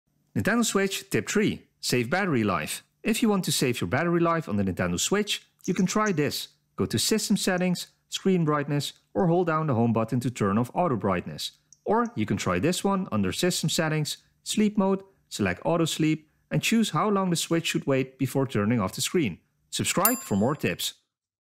Nintendo Switch tip 3 - Save battery life 🔋
Are you want to PLAY LONGER on your Nintendo Switch in handheld mode?

This tip might help you resolve it!